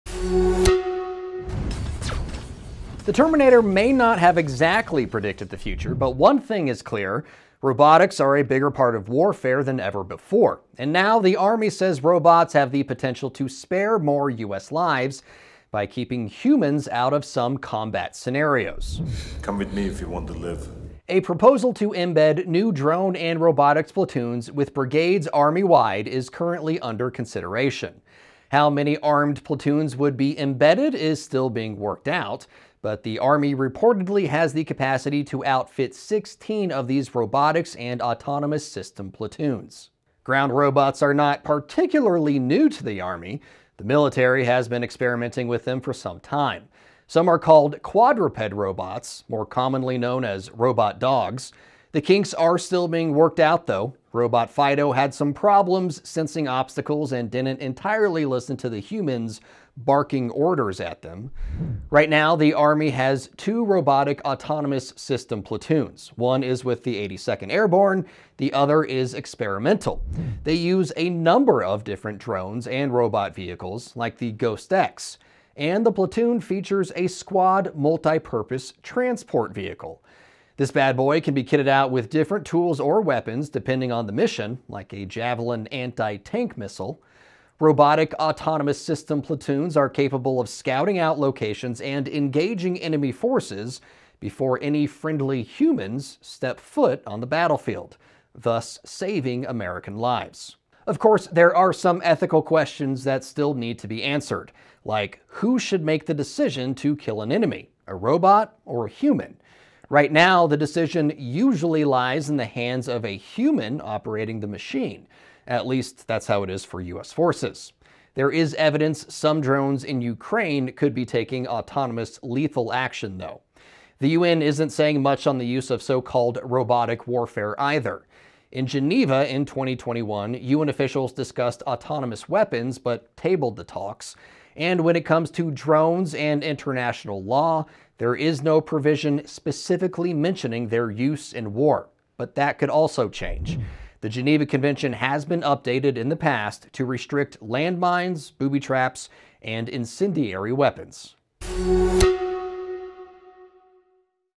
A robotic future: US Army considering a platoon of steel to save lives
A proposal to embed new drone and robotics platoons within brigades U.S. Army-wide is reportedly under consideration. The Army says the platoons have the opportunity to save lives by further limiting the direct combat for human troops.